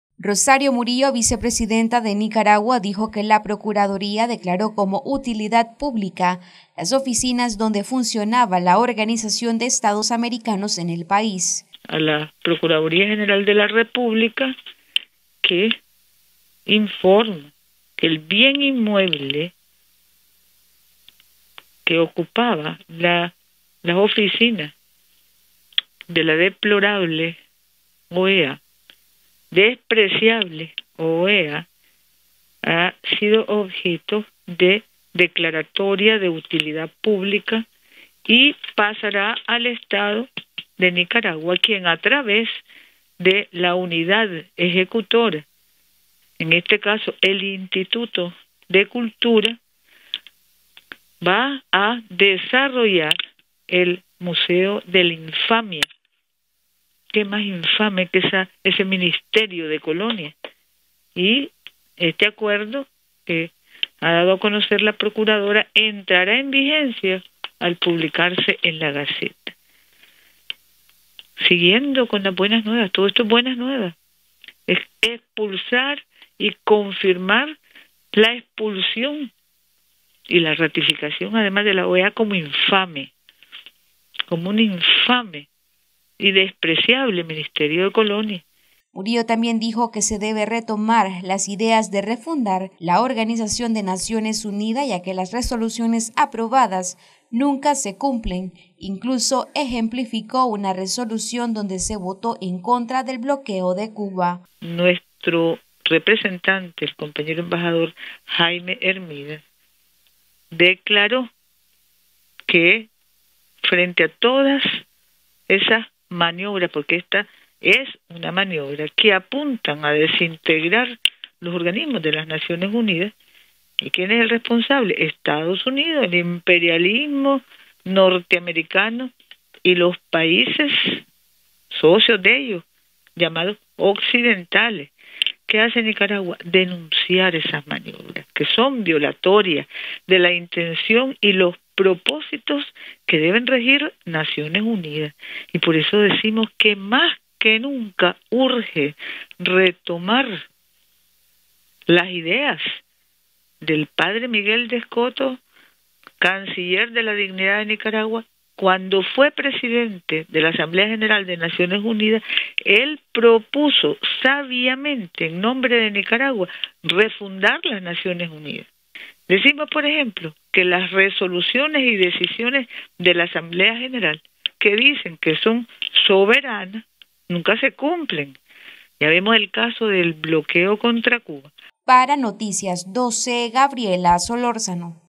Gobierno declara de utilidad pública mobiliario que perteneció a la OEA en Nicaragua
Noticias12Nicaragua Gobierno declara de utilidad pública mobiliario que perteneció a la OEA en Nicaragua, además asegura que se debe refundar la ONU ya que Resoluciones aprobadas en las Asambleas de las Naciones Unidas nunca se cumplen